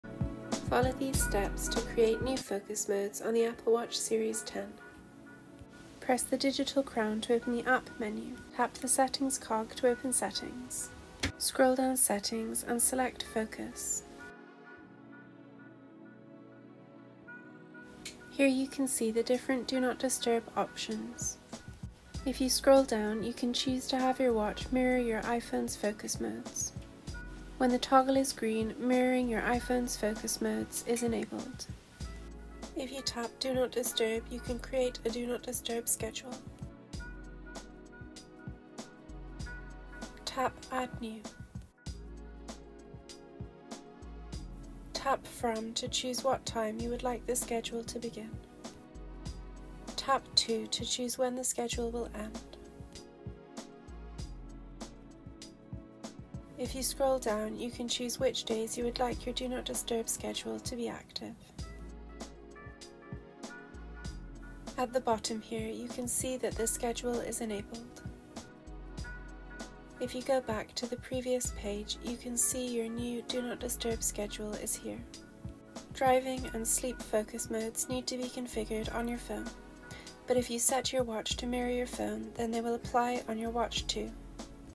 Create Focus Modes On Apple Watch Series 10
Easy to follow tutorial on creating focus modes on your Apple Watch Series 10.